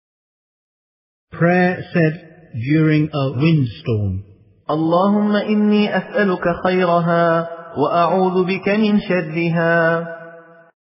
Invocation for when the wind blows
O Allah , I ask You for the good of it and seek refuge in You against its evil.